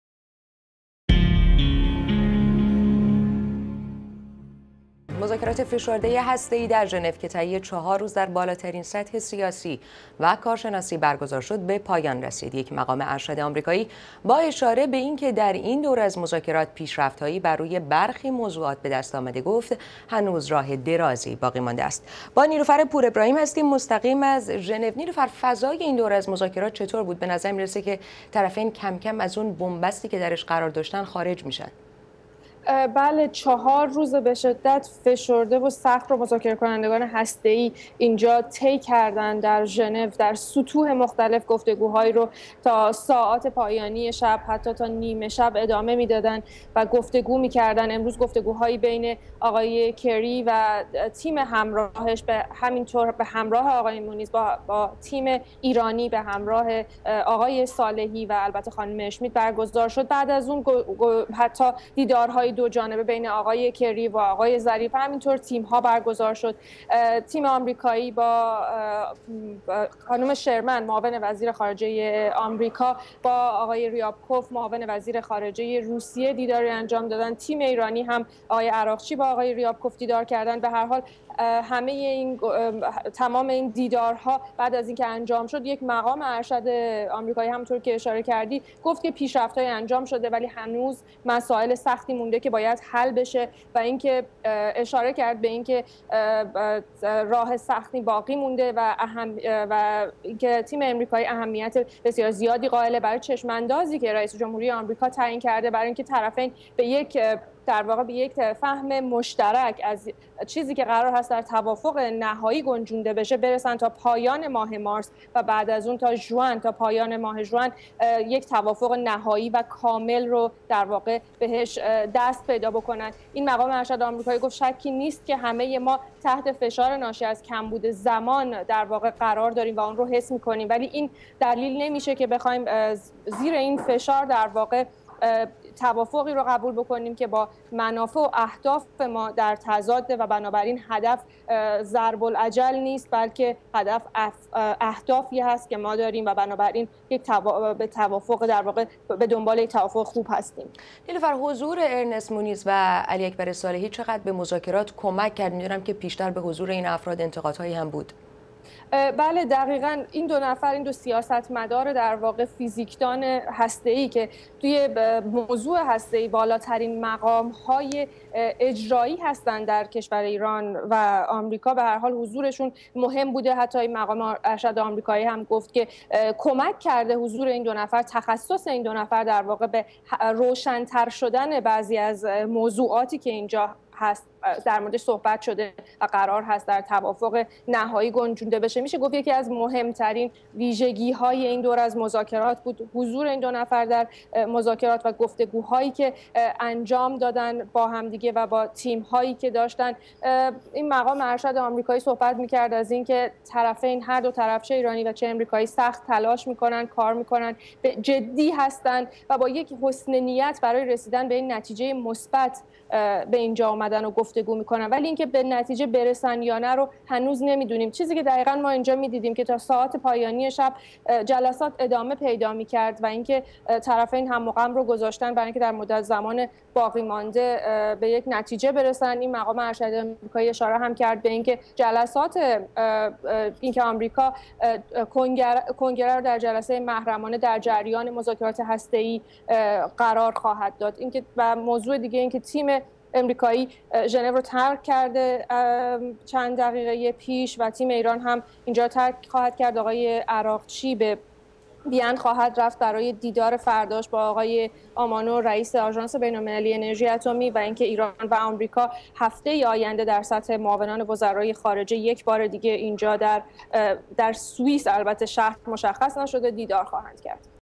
پایان چهارمین روز مذاکرات اتمی ایران در ژنو
چهارمین روز یک دور دیگر از مذاکرات اتمی ایران روز دوشنبه در ژنو پایان یافت. در این روز جان کری، وزیر خارجه آمریکا و جواد ظریف، وزیر امورخارجه ایران بار دیگر دیدار و گفتگو کردند.